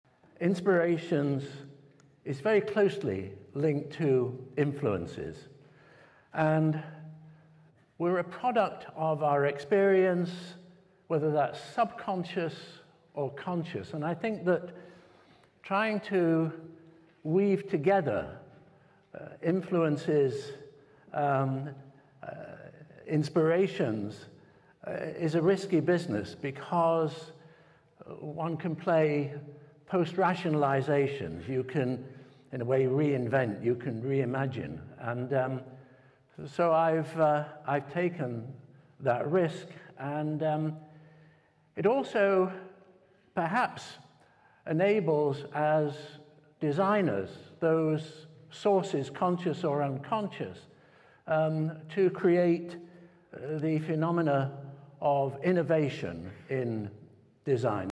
Inspirations Lecture at Manchester School of Architecture - Norman Foster
‘Inspirations is very closely linked to influences. It enables us designers tu create the phenomena of innovation’ - Norman Foster

Inspirations Lecture at the Manchester School of Architecture